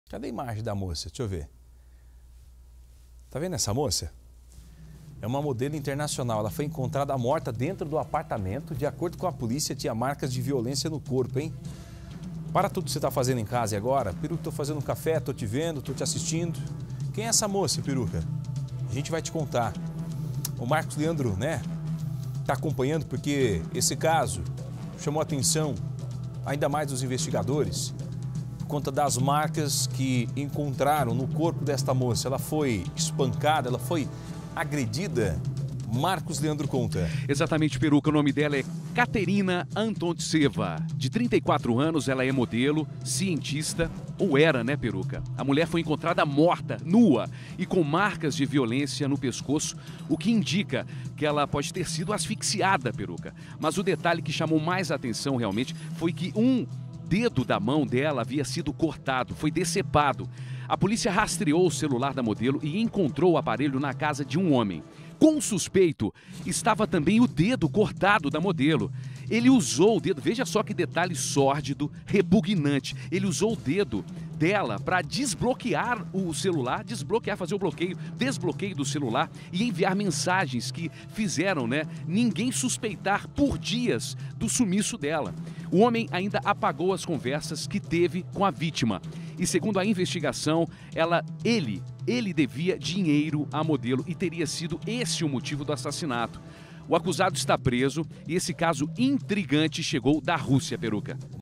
Modelo é morta e tem dedo decepado pelo assassino na Rússia
Ekaterina Antontseva, de 34 anos, foi encontrada morta dentro do apartamento dela, na Rússia. O que chamou a atenção dos policiais foi o dedo decepado da modelo.